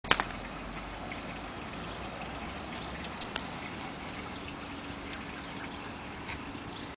Alan practically running!
3 weeks post spinal cord injury and he's getting around greyt! Still does not have full use of the left leg tho.